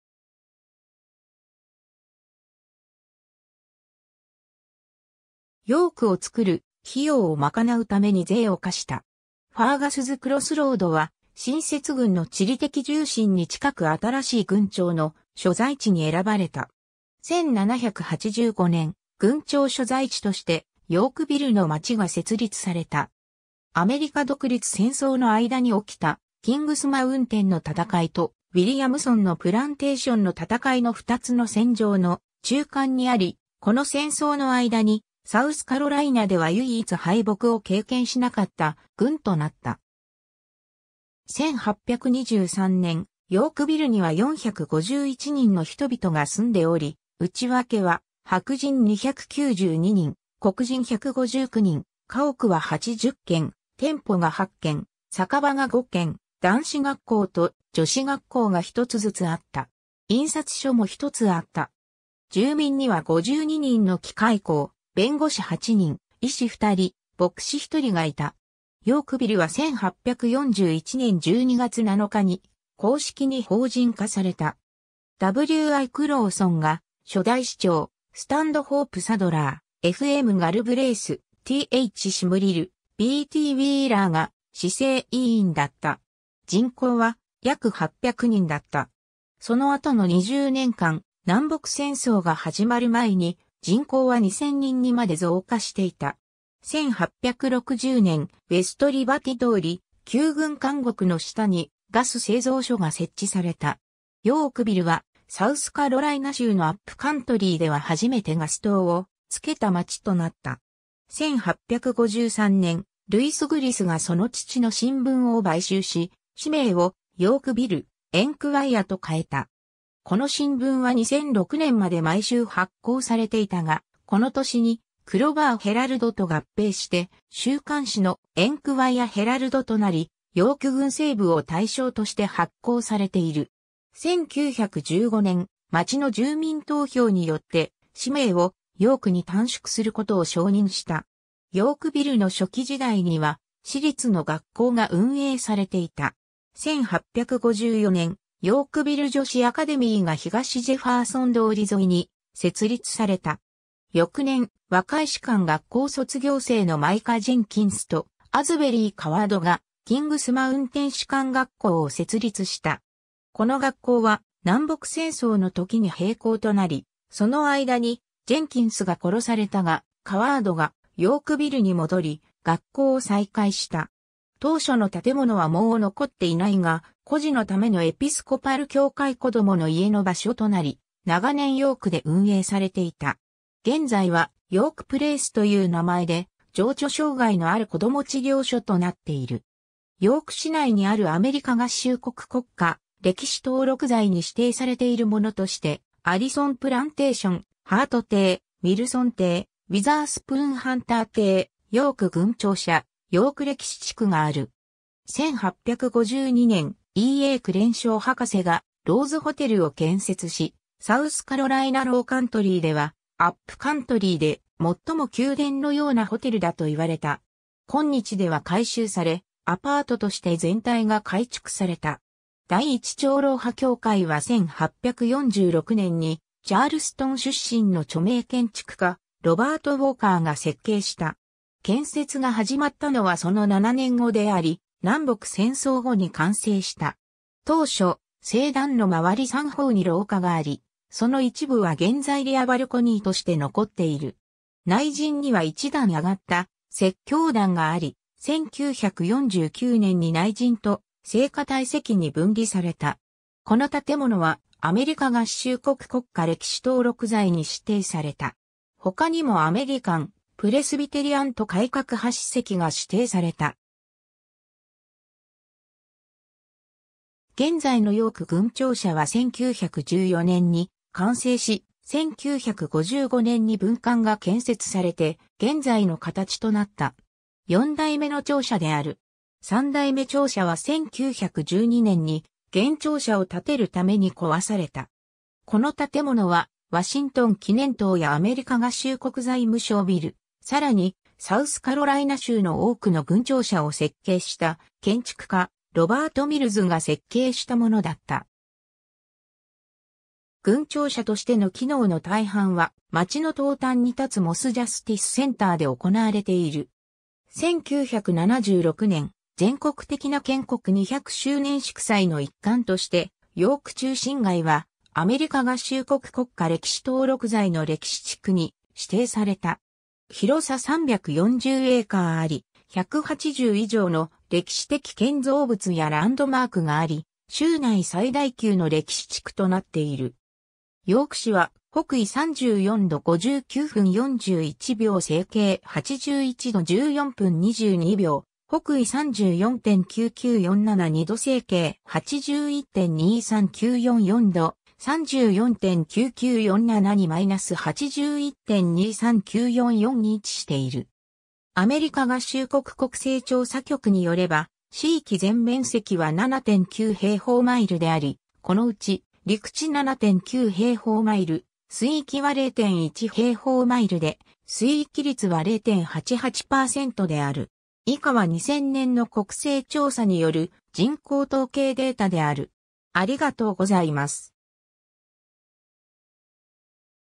ヨーク (サウスカロライナ州)
ヨーク（英: York、[ˈjɔːrk]) は、アメリカ合衆国サウスカロライナ州最北部のヨーク郡に位置する都市であり、同郡の郡庁所在地である。2010年国勢調査での人口は7,736 人だった。ノースカロライナ州との州境近くにあり、同州シャーロット市の南西約27マイル (43 km)、ヨーク郡では最大の都市ロックヒル市からは西に13マイル (21 km) に位置している。 1750年代初期、最初のヨーロッパ人開拓者がヨークの地域に入って来た。現在のペンシルベニア州やバージニア州から流れてきた。ペンシルベニアに入っていた主要な3通りの集団の中から、まずイングランド人、続いてドイツ人とスコットランド人の順で移って来た。サウスカロライナ州の郡名になっているランカスター郡、チェスター郡およびヨーク郡はイングランドの地名からペンシルベニアにもたらされたものであり、さらにサウスカロライナの初期開拓者が持ってきた。 それ以前に、ヨークの地域にまず居住していたのはカトーバ族インディアンだった。 ヨークの町は当初、ウィリアムとジョンのファーガス兄弟が所有していた酒場にちなみ、ファーガスズ・クロスロードと呼ばれていた。それはラザフォードトンからカムデンに至る道路と、シャーロッツバーグからジョージア州オーガスタに至る道路が交差する場所にあった。1785年、ヨーク郡が設立されたとき、州議会が各郡の最も便利な場所にに郡庁舎と公共の建物を建てるよう要求し、「裁判所、監獄、さらし台、むち打ち柱、ストック（一種のさらし台）を作る」費用を賄うために税を課した。
ファーガスズ・クロスロードは新設郡の地理的重心に近く新しい郡庁の所在地に選ばれた。
 1785年、郡庁所在地としてヨークビルの町が設立された。
アメリカ独立戦争の間におきたキングスマウンテンの戦いと、ウィリアムソンのプランテーションの戦いの2つの戦場の中間にあり、この戦争の間にサウスカロライナでは唯一敗北を経験しなかった郡となった。
 1823年、ヨークビルには451人の人々が住んでおり、内訳は白人292人、黒人159人、家屋は80軒、店舗が8軒、酒場が5軒、男子学校と女子学校が1つずつあった。
印刷所も1つあった。
住民には52人の機械工、弁護士8人、医師2人、牧師1人がいた。
 ヨークビルは1841年12月7日に公式に法人化された。
W・I・クローソンが初代市長、スタンドホープ・サドラー、F・M・ガルブレイス、T・H・シムリル、B・T・ウィーラーが市政委員だった。
人口は約800人だった。
その後の20年間、南北戦争が始まる前に、人口は2,000人にまで増加していた。
1860年、ウェストリバティ通り、旧郡監獄の下にガス製造所が設置された。
ヨークビルはサウスカロライナ州のアップカントリーでは初めてガス灯を付けた町となった。
 1853年、ルイス・グリスがその父の新聞を買収し、紙名を「ヨークビル・エンクワイア」と変えた。
この新聞は2006年まで毎週発行されていたが、この年に「クロバー・ヘラルド」と合併して週刊紙の「エンクワイア・ヘラルド」となり、ヨーク郡西部を対象として発行されている。
 1915年、町の住民投票によって、市名を「ヨーク」に短縮することを承認した。
 ヨークビルの初期時代には私立の学校が運営されていた。
1854年、ヨークビル女子アカデミーが東ジェファーソン通り沿いに設立された。
翌年、若い士官学校卒業生のマイカ・ジェンキンスとアズベリー・カワードが、キングスマウンテン士官学校を設立した。
この学校は南北戦争の時に閉校となり、その間にジェンキンスが殺されたが、カワードがヨークビルに戻り、学校を再開した。
当初の建物はもう残っていないが、孤児のためのエピスコパル教会子供の家の場所となり、長年ヨークで運営されていた。
現在はヨーク・プレースという名前で、情緒障害のある子供治療所となっている。
 ヨーク市内にあるアメリカ合衆国国家歴史登録財に指定されているものとして、アリソン・プランテーション、ハート邸、ウィルソン邸、ウィザースプーン・ハンター邸、ヨーク郡庁舎、ヨーク歴史地区がある。
 1852年、E・A・クレンショー博士がローズ・ホテルを建設し、サウスカロライナ・ローカントリーでは、アップカントリーで最も宮殿のようなホテルだと言われた。
今日では改修され、アパートとして全体が改築された。
 第一長老派教会は1846年にチャールストン出身の著名建築家ロバート・ウォーカーが設計した。
建...